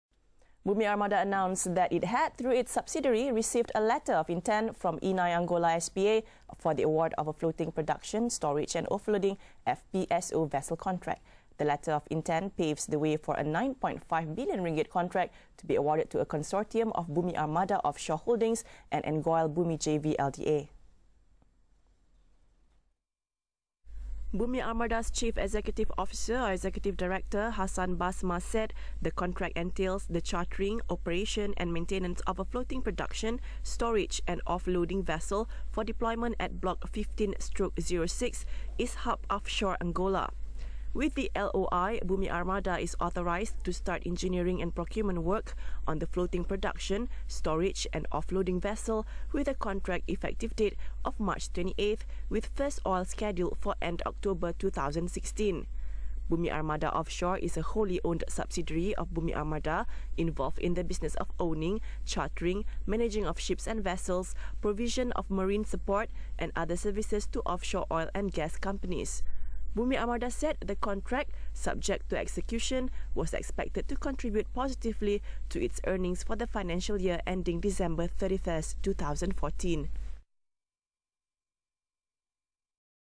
Bumi Armada: FPSO contract worth RM9.58
Bumi Armada announced that it had, through its subsidiary, received a letter of intent from Eni Angola SPA for the award of a Floating Production, Storage and Offloading (FPSO)  vessel contract.

The letter of intent paves the way for a RM9.5 billion contract to be awarded to a consortium of Bumi Armada Off Shore Holdings  and Angoil Bumi JVLDA.